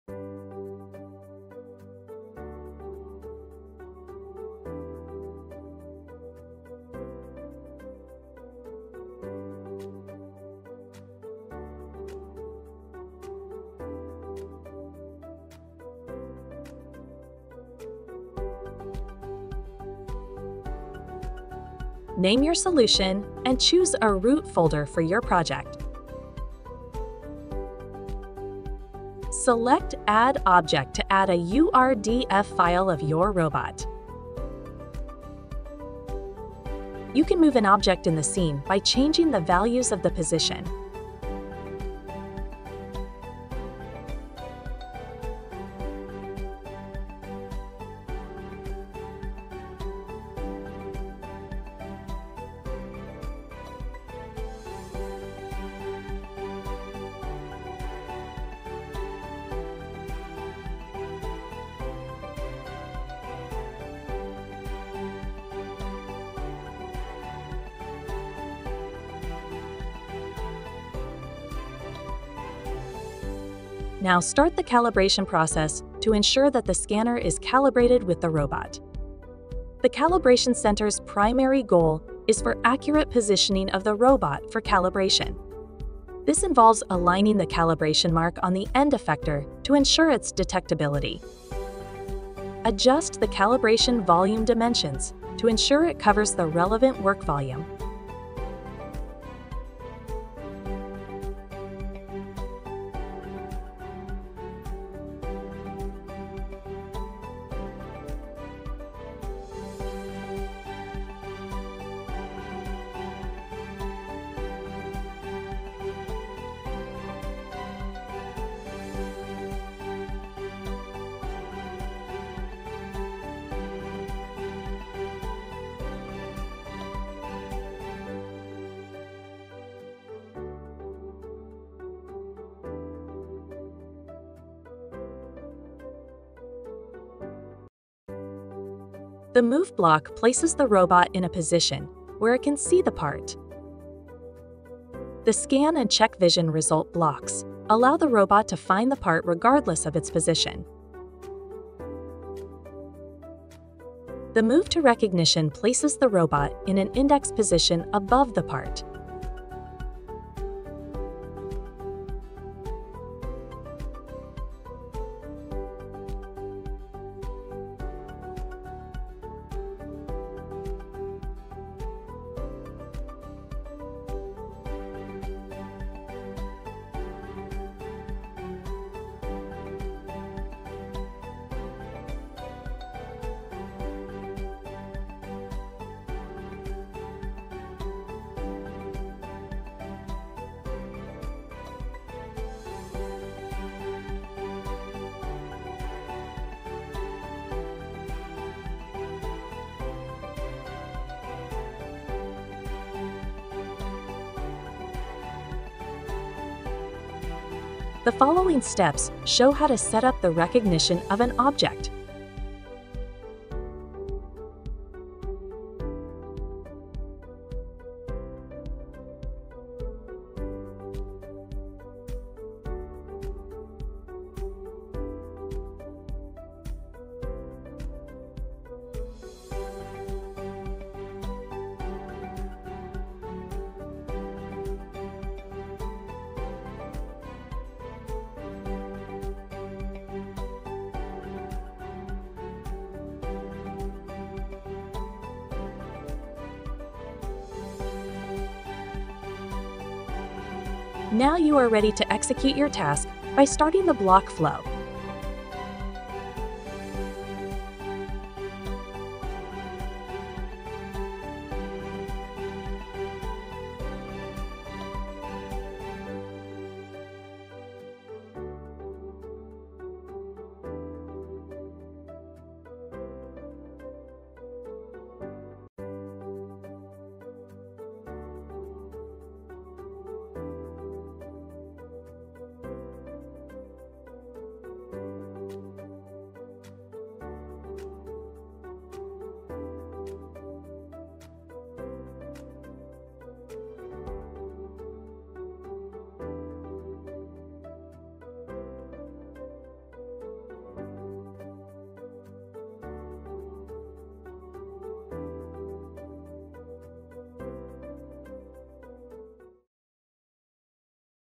Pick and Place Solution With SCAPE CoCreator
This video demonstrates how easy it is to create a pick-and-place solution using SCAPE CoCreator. The SCAPE CoCreator streamlines development, merging digital planning with physical execution. It is designed for users of all skill levels, enabling the creation of robotic automation applications without extensive technical know-how. This platform embodies our legacy in robotic guidance and our dedication to making industrial automation more accessible, adaptable, and resource-efficient. For this case example, a FANUC robot was used. However, the CoCreator is compatible with a wide range of robot brands.

0:00 Introduction
0:17 Create a Solution
0:27 Build Your Scene
1:02 Selecting Hardware
1:11 Calibration
2:13 Building a Block Flow
3:23 Object Recognition
4:09 Block Flow Running